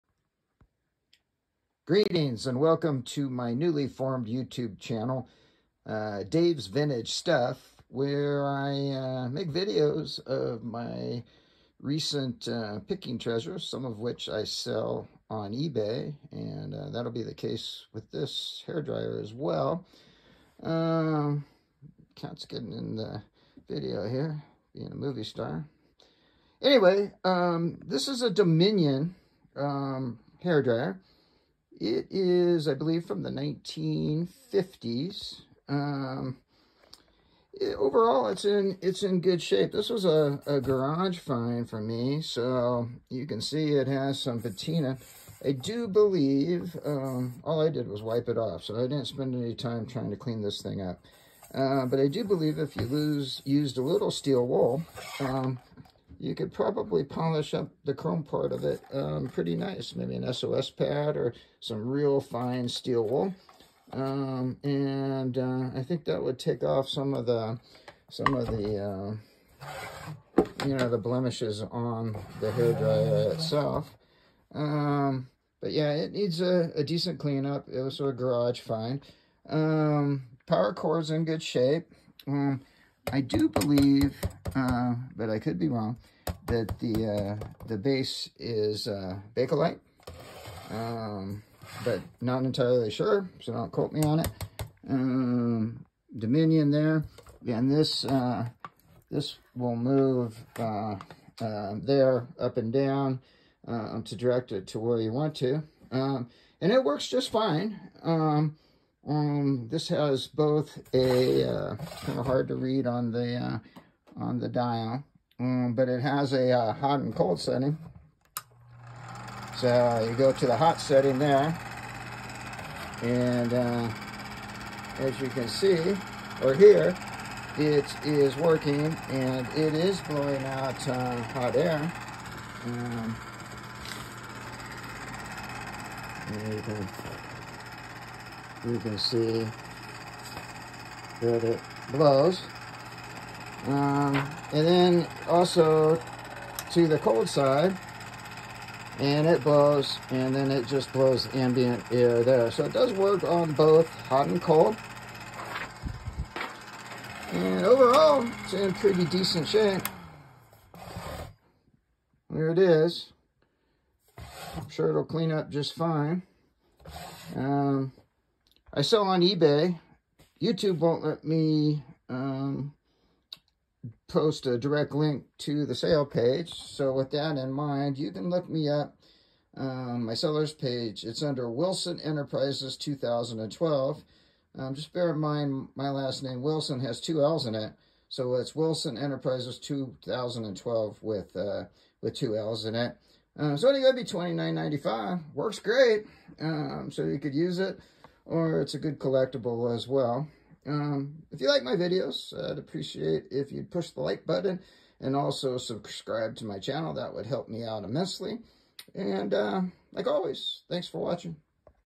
Vintage 1950’s Dominion Hair Dryer
Vintage Mid Century Modern Dominion Hair Dryer With Base. Works Great. Much appreciated.